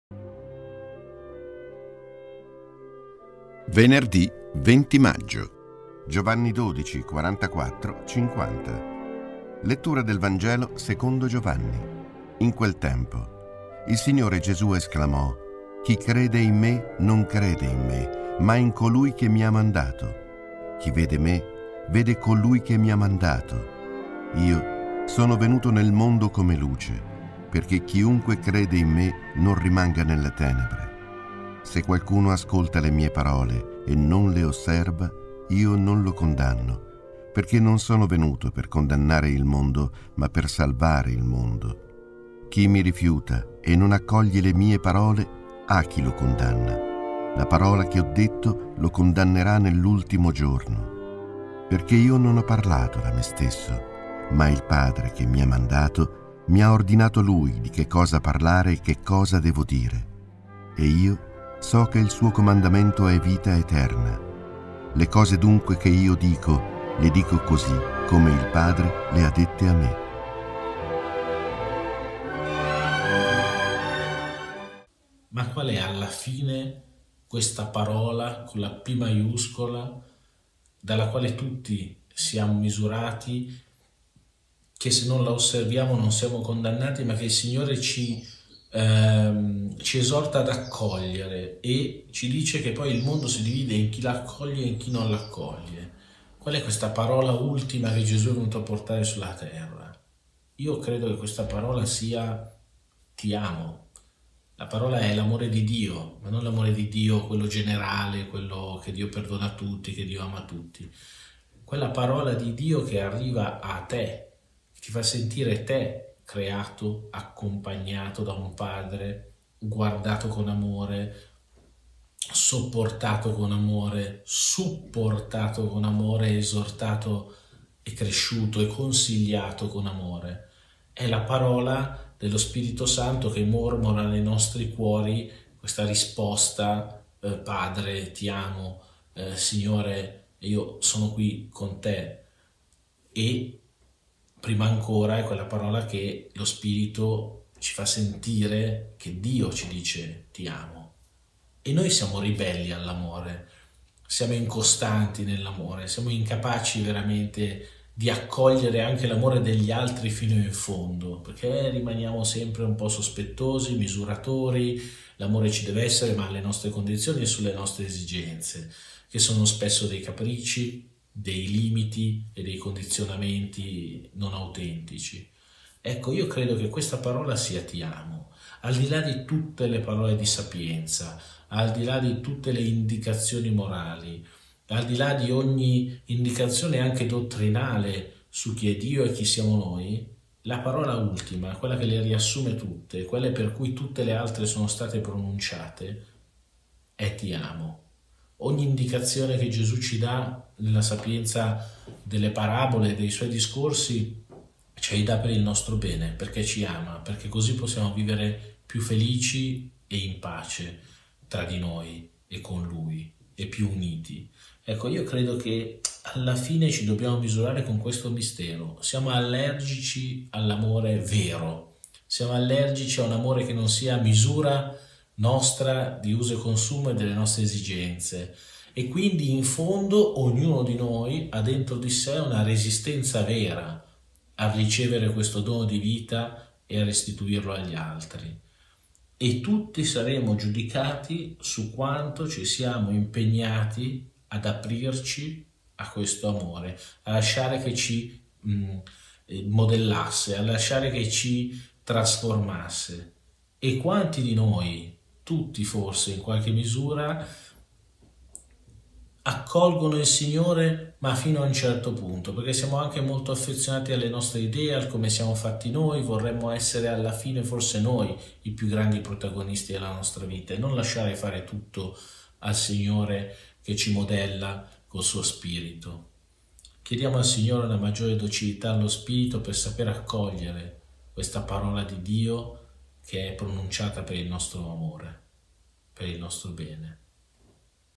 Vangelo del giorno venerdì 20 maggio 2022 commenti di don Giacomo Caprio
Gv 15,12-17

In quel tempo, disse Gesù ai suoi discepoli:
«Questo è il mio comandamento: che vi amiate gli uni gli altri come io ho amato voi. Nessuno ha un amore più grande di questo: dare la sua vita per i propri amici.

Voi siete miei amici, se fate ciò che io vi comando. Non vi chiamo più servi, perché il servo non sa quello che fa il suo padrone; ma vi ho chiamati amici, perché tutto ciò che ho udito dal Padre mio l'ho fatto conoscere a voi.

Non voi avete scelto me, ma io ho scelto voi e vi ho costituiti perché andiate e portiate frutto e il vostro frutto rimanga; perché tutto quello che chiederete al Padre nel mio nome, ve lo conceda. Questo vi comando: che vi amiate gli uni gli altri».